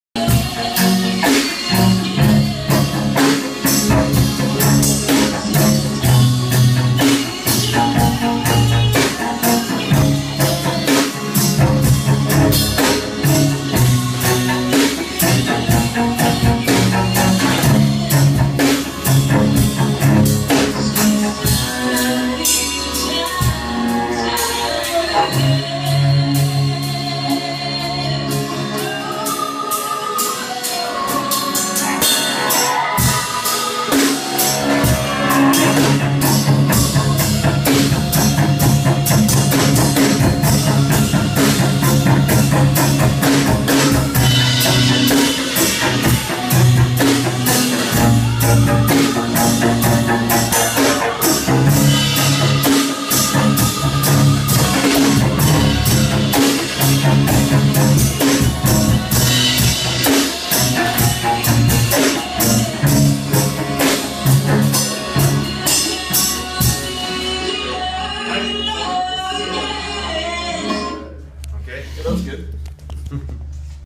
Sandi Thom rehearses with her band via Skype
Harrah for technology guys, we rehearsed today over Skype! The lads were in Coatbridge, Scotland and I was in Los Angeles - We are gearing up for this weekends Burns an' a' That festival in Ayr. We sound alright considering we are in different countries!

Don't worry everyone I will be in person come Saturday! Who's all coming? For more info on this FREE show

Vocals: Sandi Thom
Drums : Scott Cowie
Cello: Calum Ingram